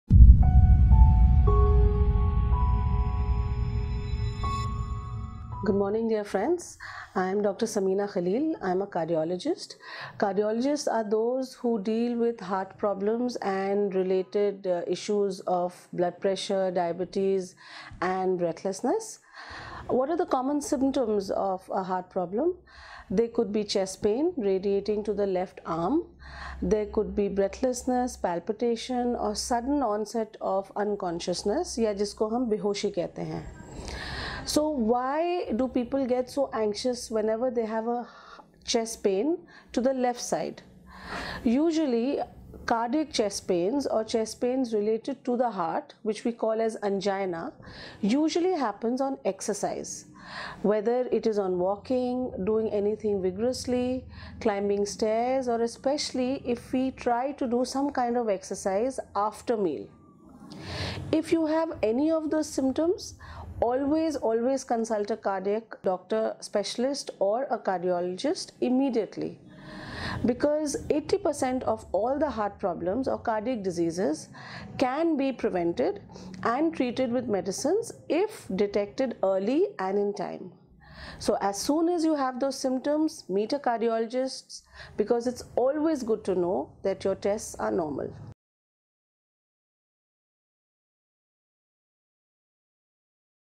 Lybrate Dr Sameena Khalil Talks About Symptoms Of Heart Problems
Few common symptoms of heart problems include chest pain radiating to the left arm, breathlessness, palpitation or unconsciousness and so on.